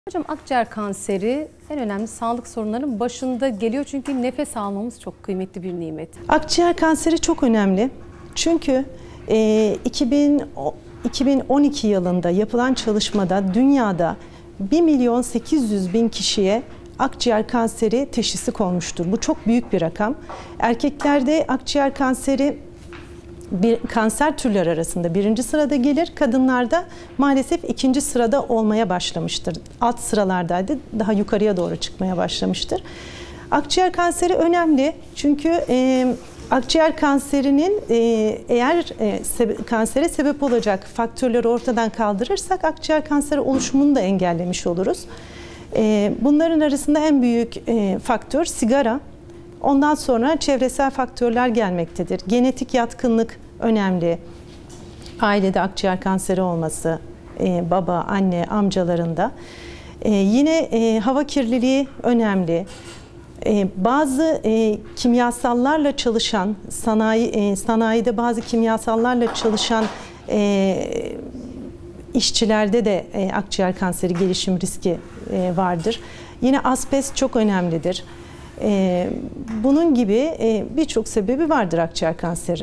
Akciğer Kanseri Nedir ? Belirtileri Ve Tedavisi Nedir ?
Avrasya Hastanesi Radyasyon Onkoloji Uzmanı Züleyha Kadehçi '' Akciğer Kanseri Nedir ? Belirtileri Ve Tedavisi Nedir ? '' hakkında detaylı bilgiler veriyor.